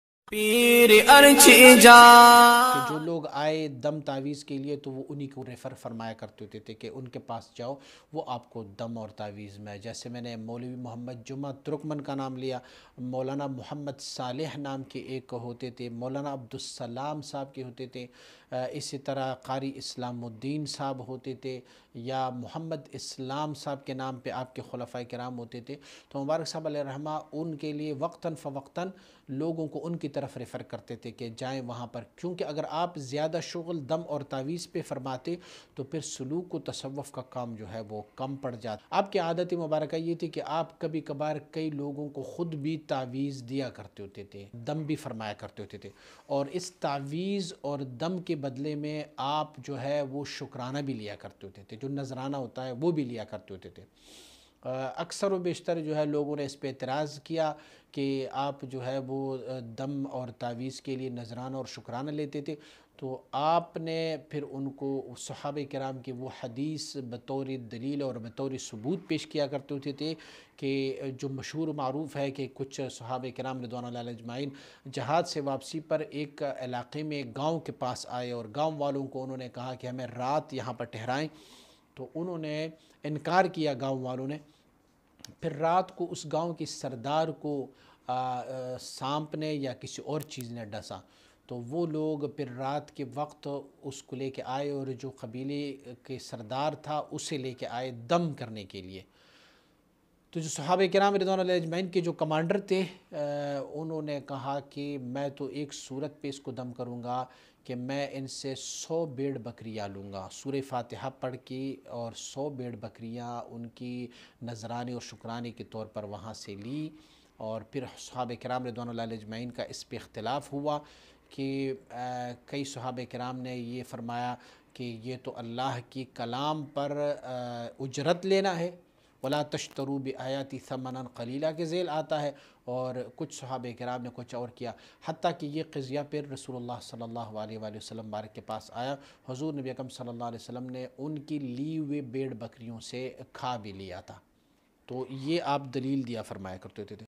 معمولاتِ مبارک صاحب : کبھی کبھار آپ خود بھی تعویذ دیتے اور اس پر اجرت بھی لیتے تھے
* بے شک انعام یافتہ لوگوں کی پیروی میں ہم لوگوں کی نجات اور عافیت ہے.

* پیر طریقت رہبر شریعت حضرت اخندزادہ ابو حبیب سیف الرحمن المعروف مبارک صاحب کے معمولات پیش خدمت ہیں.
*معمولات مبارک صاحب خاص طور پر سلسلہ عالیہ سیفیہ کے وابستگان کے لیے پیش کیے جا رہے ہیں تاکہ وہ اپنے مرشد کے قدم پر قدم رکھیں اور جو اعمال و افعال و عبادت و ریاضت کے طریقے پر مبارک صاحب چل رہے تھے ہم لوگ بھی اس کو اپنائیں.

*آپ کے تمام معمولات کو قرآن و حدیث اور صالحین کی تعلیمات سے ثابت بھی کیا جائے گا.

✓مزید معمولات مبارک صاحب دیکھنے کے لئے نیچے دیئے گئے پلے لسٹ کے لنک پر کلک کریں

Speaker : Sahibzada Ahmed Saeed Yar jaan saifi sahib S/O Akundzada Pir Saifurehman Mubarik Sahib From Markazi Astana Aliya Safia Lahore, Pakistan